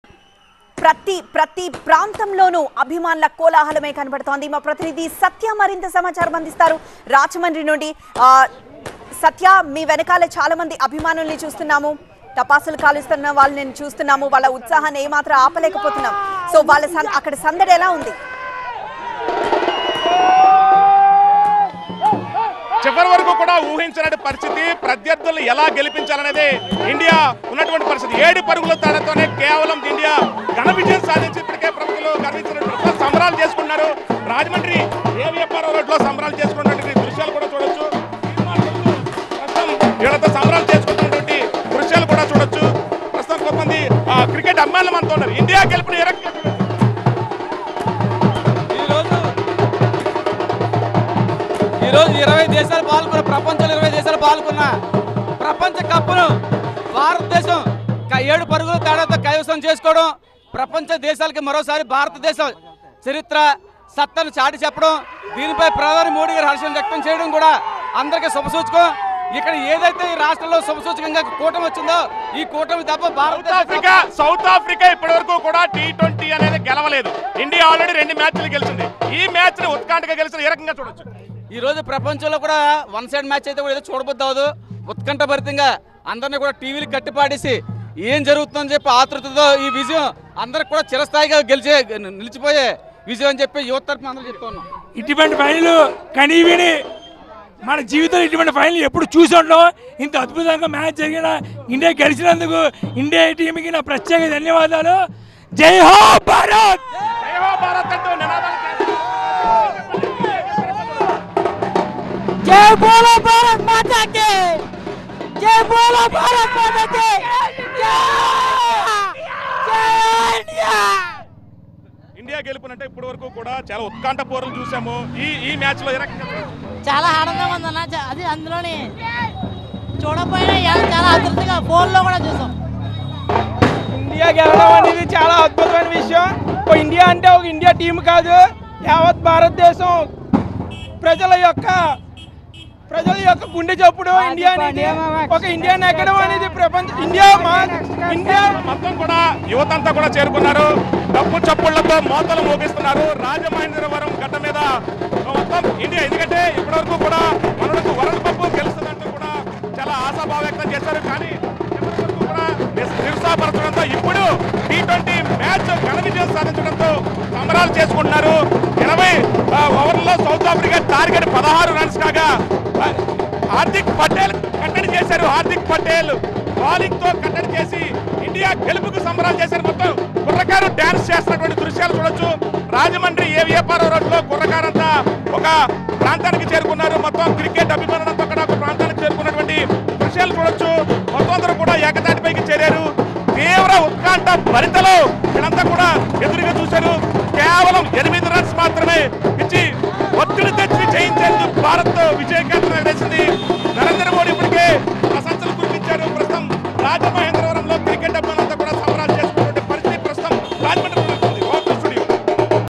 T20 World Cup 2024 : భారత్‌ ఘన విజయం... అభిమానుల కోలాహలం - TV9
Credits